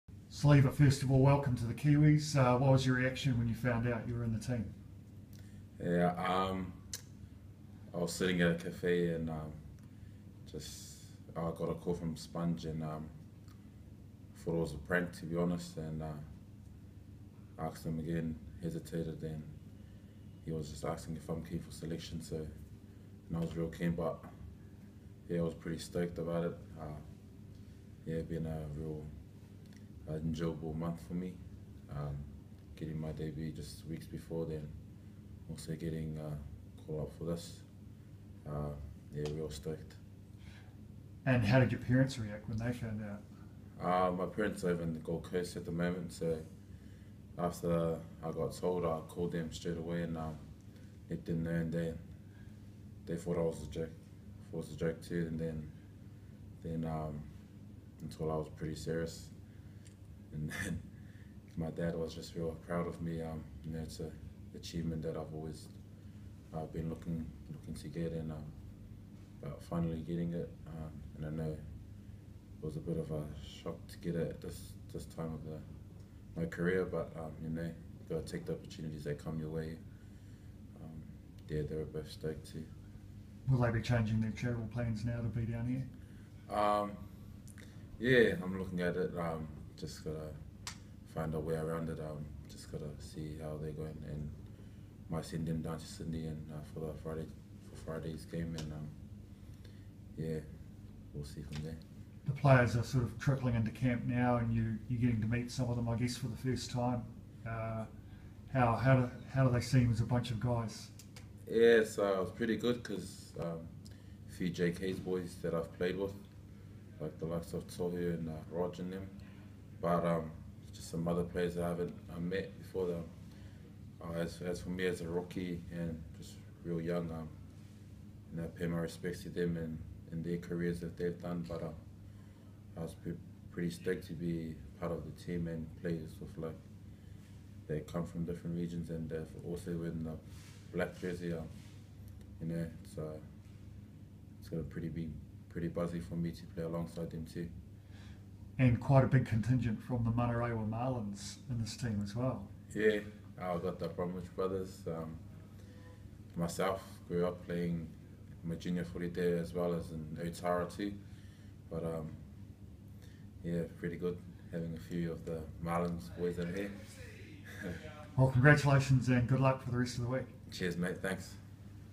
Happy hooker Havili surprised at Kiwis call-up
NZ Warriors hooker Siliva Havili recalls how he found out about his Kiwis selection for the 2014 Anzac Test